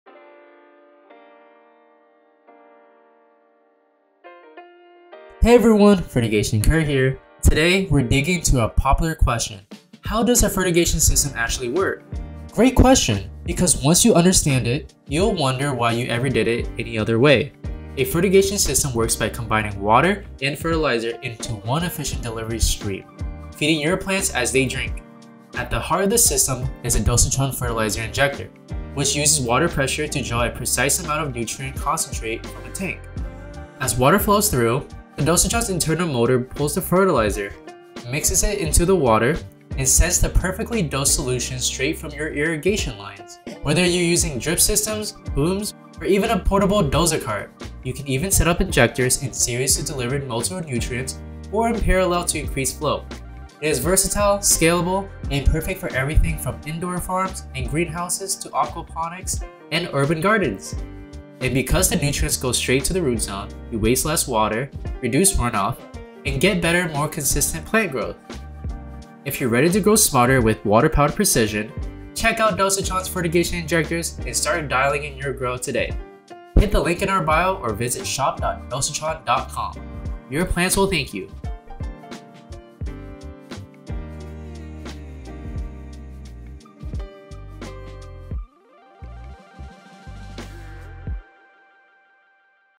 How Does a Fertigation System Work? 💧 Fertigation Explained in 60 Seconds 🌱
💡 𝐇𝐨𝐰 𝐝𝐨𝐞𝐬 𝐚 𝐟𝐞𝐫𝐭𝐢𝐠𝐚𝐭𝐢𝐨𝐧 𝐬𝐲𝐬𝐭𝐞𝐦 𝐚𝐜𝐭𝐮𝐚𝐥𝐥𝐲 𝐰𝐨𝐫𝐤?
Fertigation Kurt breaks it down in simple terms so you can see why it’s the smarter way to feed your plants.

💧 𝐓𝐡𝐞 𝐏𝐫𝐨𝐜𝐞𝐬𝐬:
A fertigation system combines fertilizer + irrigation into one stream. At the center is the Dosatron fertilizer injector, which uses water pressure to pull nutrients from a concentrate tank, mix them with water, and deliver a perfectly dosed solution through your irrigation lines.

🌱 𝐊𝐞𝐲 𝐅𝐞𝐚𝐭𝐮𝐫𝐞𝐬 𝐨𝐟 𝐅𝐞𝐫𝐭𝐢𝐠𝐚𝐭𝐢𝐨𝐧 𝐒𝐲𝐬𝐭𝐞𝐦𝐬:
- 𝐖𝐚𝐭𝐞𝐫-𝐩𝐨𝐰𝐞𝐫𝐞𝐝 – No electricity needed
-  𝐀𝐜𝐜𝐮𝐫𝐚𝐭𝐞 𝐝𝐨𝐬𝐢𝐧𝐠 – Consistent nutrient ratios every time
- 𝐅𝐥𝐞𝐱𝐢𝐛𝐥𝐞 𝐬𝐞𝐭𝐮𝐩𝐬 – Works with drip irrigation, sprinklers, booms, and even portable carts
- 𝐒𝐜𝐚𝐥𝐚𝐛𝐥𝐞 – Set injectors in series for multiple nutrients or in parallel for higher flow
- 𝐄𝐜𝐨-𝐟𝐫𝐢𝐞𝐧𝐝𝐥𝐲 – Reduce runoff and fertilizer waste

From indoor farms and greenhouses to aquaponics and urban gardens, fertigation ensures plants get exactly what they need at the root zone.